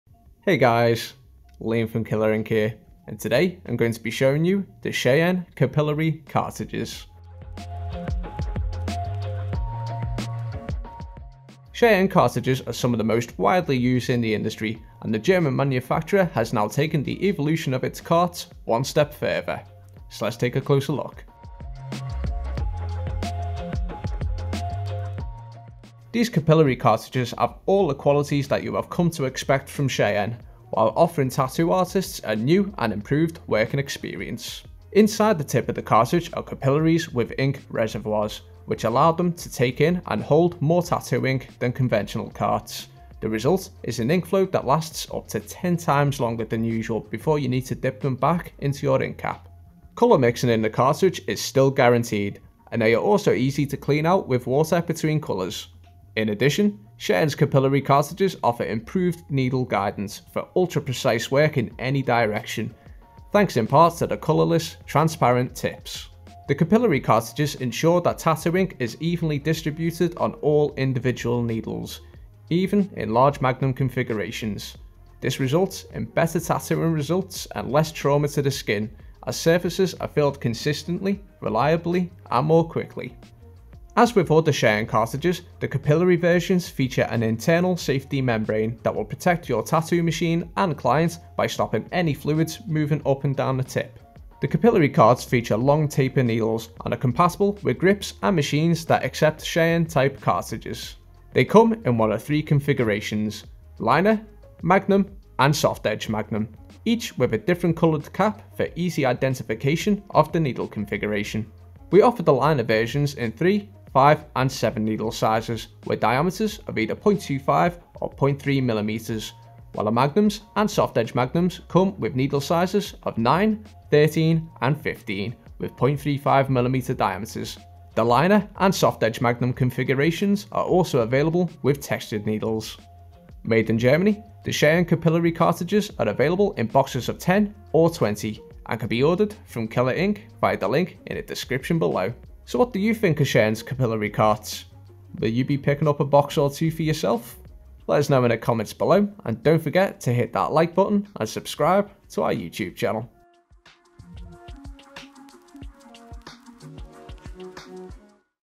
Cheyenne Capillary Cartridges | Review, Setup & Unboxing
We unbox, set-up and review the Cheyenne Capillary Cartridges and talk you through its features.

Cheyenne's Capillary Cartridges offer tattoo artists a new and improved working experience.

Inside the tip of the cartridge are capillaries with ink reservoirs, which allow the cartridges to absorb and hold more tattoo ink than conventional cartridges.

The result is an ink flow that lasts up to 10 times longer than usual before needing to be dipped into your ink cap.

The Cheyenne Capillary Cartridges ensure that tattoo ink is evenly distributed on all individual needles, even in large magnum configurations. This results in better tattooing results and less trauma to the skin, as surfaces are filled consistently, reliably and more quickly.

Cheyenne Capillary Cartridges are available to order from Killer Ink Tattoo

Hey guys, Liam from Killer Ink here, and today I’m going to be showing you the Cheyenne Capillary Cartridges.

Cheyenne cartridges are some of the most widely-used in the industry and the German manufacturer has now taken the evolution of its carts one step further.

So let’s take a closer look…

These Capillary Cartridges have all the qualities that you have come to expect from Cheyenne, while offering tattoo artists a new and improved working experience.

Inside the tip of the cartridge are capillaries with ink reservoirs, which allow them take in and hold more tattoo ink than conventional carts. The result is an ink flow that lasts up to 10 times longer than usual before you need to dip them back into your ink cap.

Colour mixing in the cartridge is still guaranteed and they are also easy to clean out with water between colours. In addition, Cheyenne’s Capillary Cartridges offer improved needle guidance for ultra-precise work in any direction, thanks in part to the colourless, transparent tips.

The Capillary Cartridges ensure that tattoo ink is evenly distributed on all individual needles, even in large magnum configurations. This results in better tattooing results and less trauma to the skin, as surfaces are filled consistently, reliably and more quickly.

As with other Cheyenne cartridges, the Capillary versions feature an internal safety membrane that will protect your tattoo machine and client by stopping any fluids moving up and down the tip.

The Capillary carts feature long taper needles and are compatible with grips and machines that accept Cheyenne-type cartridges. They come in one of three configurations: Liner, Magnum, and Soft-Edge Magnum, each with a different coloured cap for easy identification of the needle configuration.

We offer the Liner versions in 03, 05 and 07 needle sizes with diameters of either 0.25 or 0.30 millimetres, while the Magnums and Soft-Edge Magnums come with needle sizes of 09, 13 and 15 with 0.35 millimetre diameters. The Liner and Soft-Edge Magnum configurations are also available with textured needles.

So what do you think of Cheyenne’s Capillary Carts? Will you be picking up a box or two for yourself?